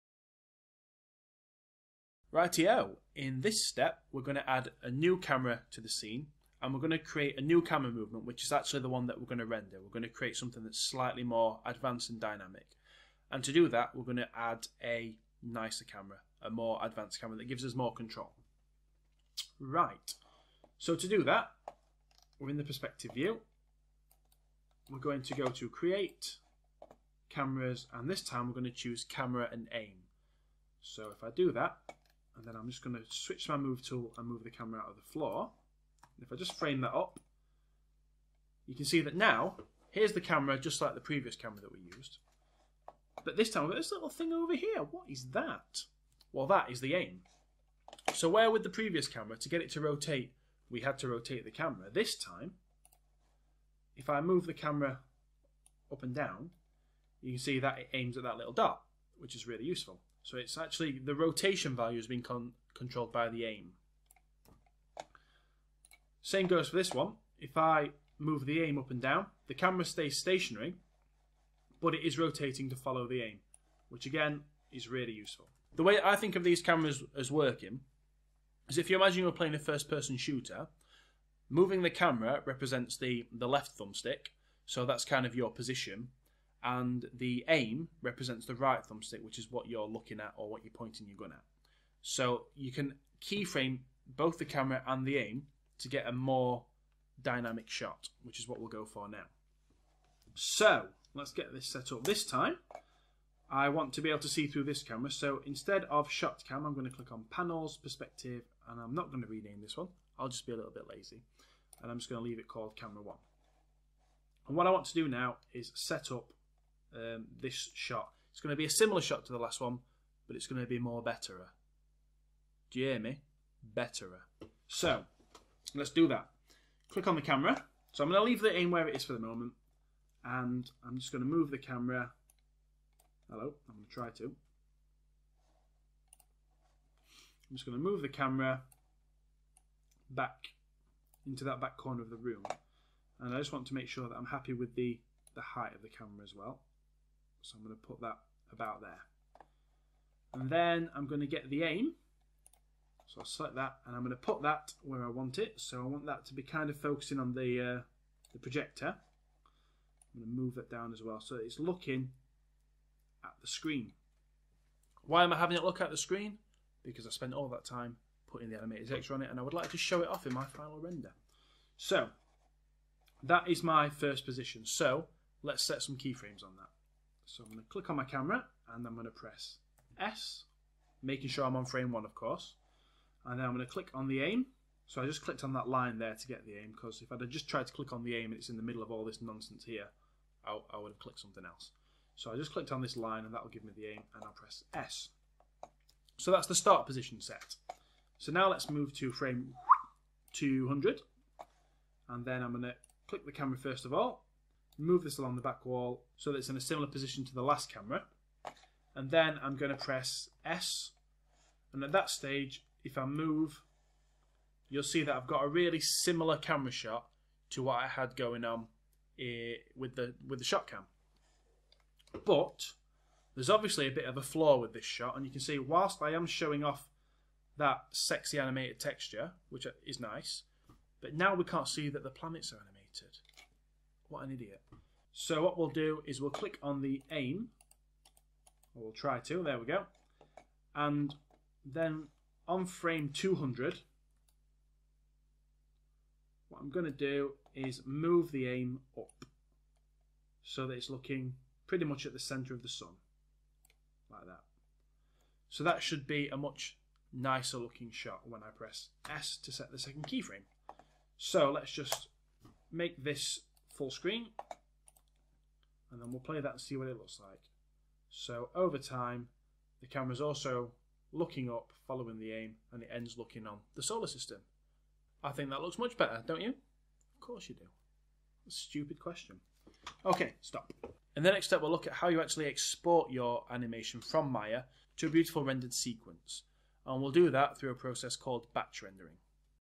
#13 Animating a camera and aim in Maya 2016 | Animation Tutorial
In this part of my Maya 2016 animation tutorial for beginners we'll take a look at what you need to do to animate a more interesting camera shot using the camera and aim.

This tutorial continues directly from my Maya 2016 for beginners tutorial.
To download the project folder to allow you to work on this tutorial without completing the previous tutorial
If you did complete the previous tutorial you'll still need the image sequence for the animated texture.